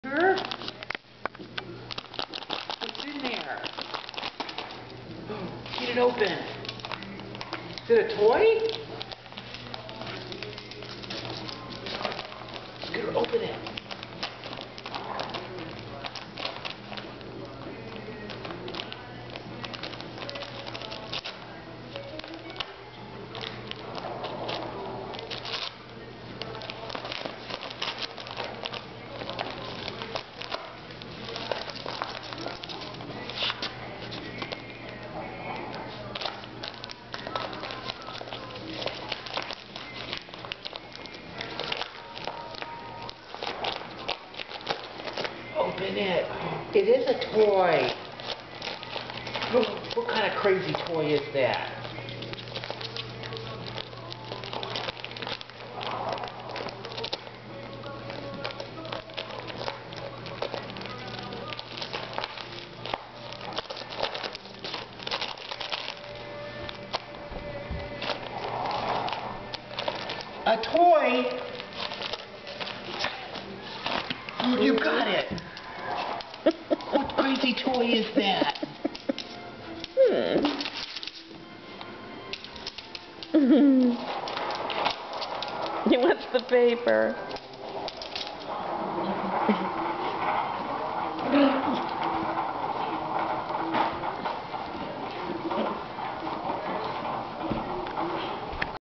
Scooter's Christmas
See Scooter open his Christmas present.